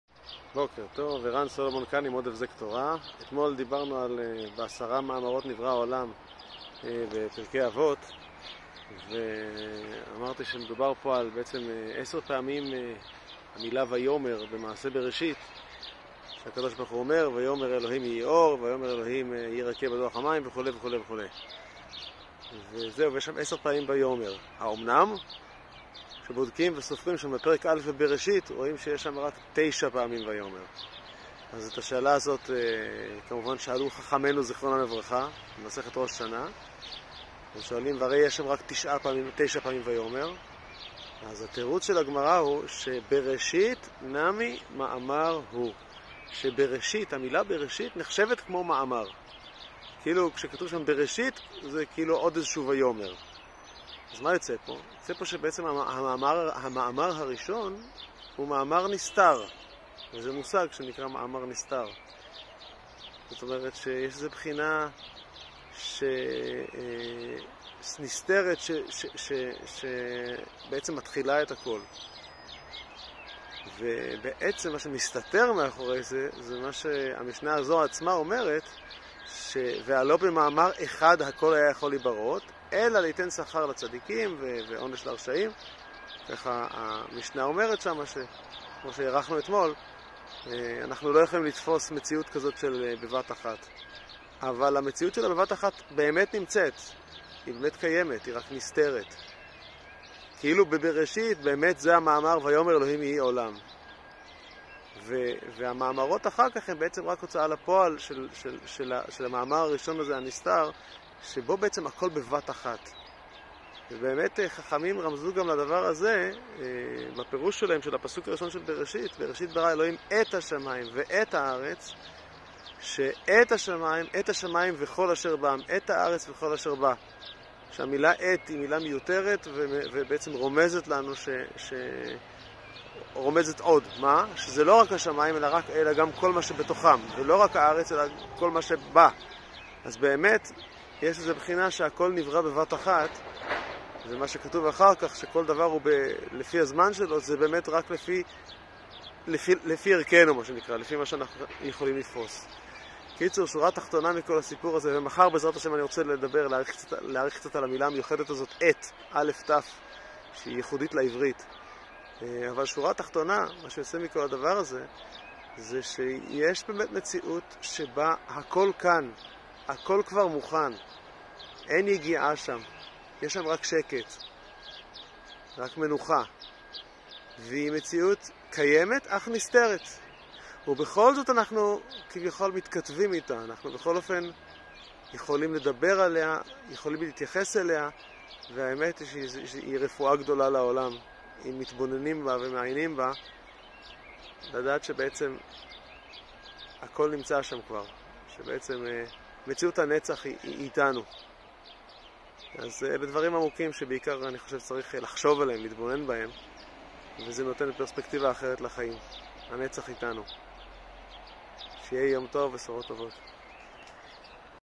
בעשרה מאמרות נברא העולם - חלק ב' - מאמר הנסתר (הבזקי תורה)
כשמסתכלים בפרק א' בבראשית רואים שיש רק 9 פעמים "ויאמר אלקים" וזו שאלה ששואל התלמוד ומשיב.
תובנה גדולה יוצאת מזה, שהיא רפואה לעולם.

הבזקי תורה=קצרצרי תורה מדי בקר מקסימום 5 דק'

מוזמנים להצטרף!